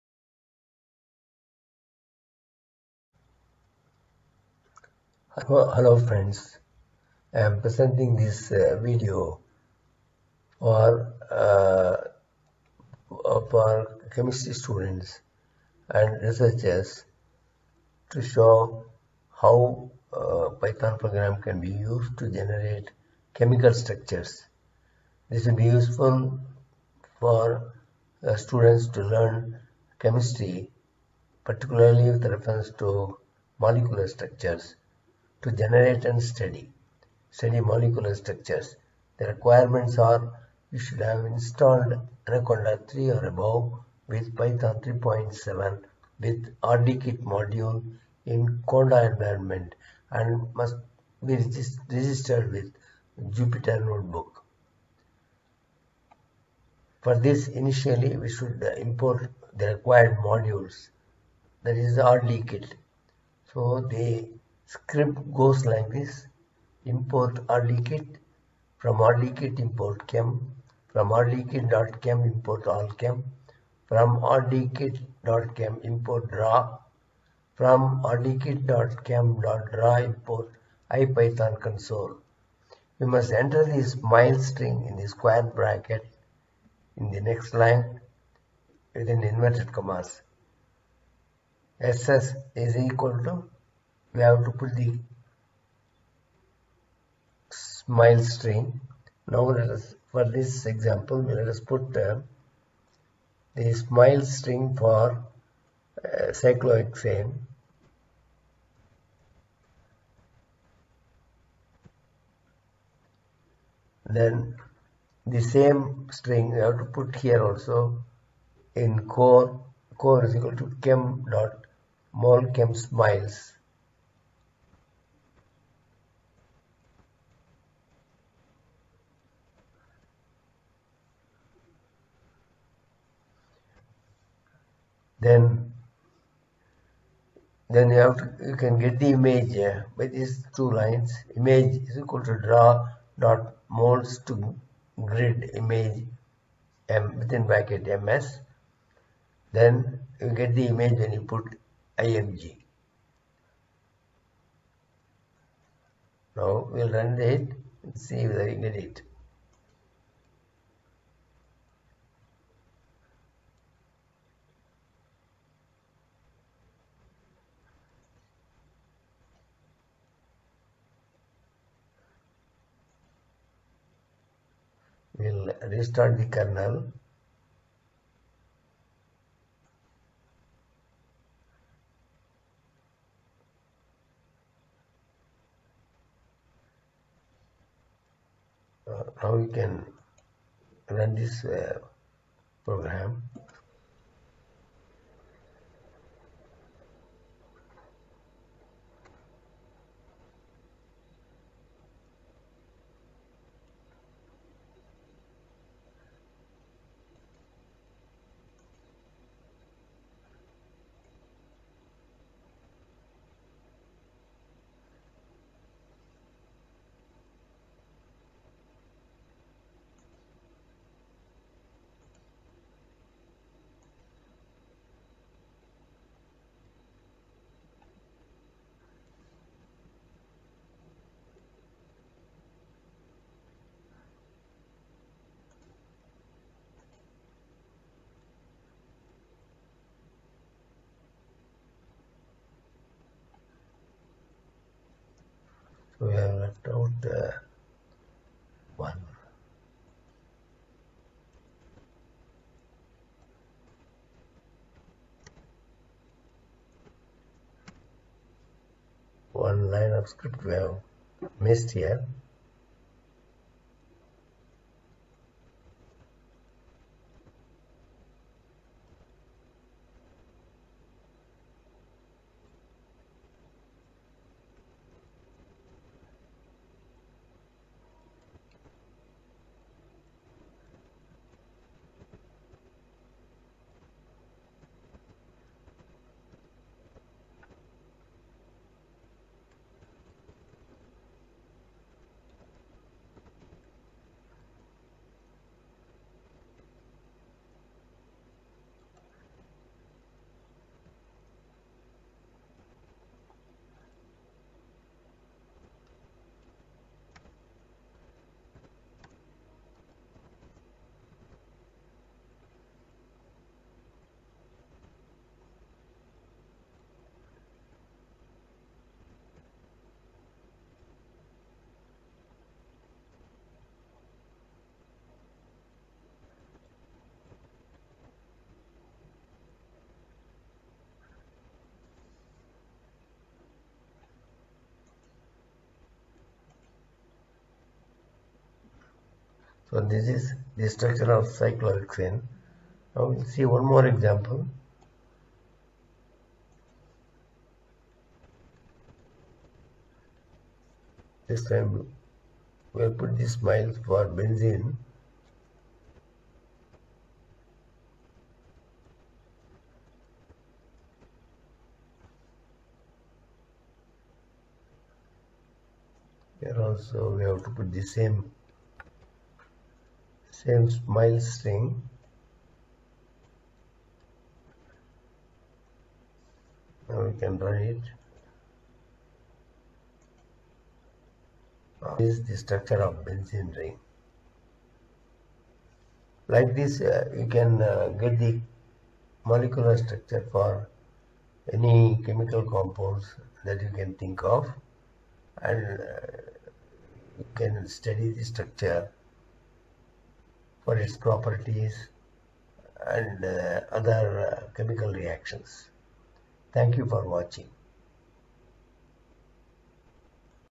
chemicalstructure in python rdkit
This short vedio explains with examples how to generate the structure of a chemical compound in python rdkit environment in jupyter notebook.This vedio will be useful to students of Chemistry and those who are interested.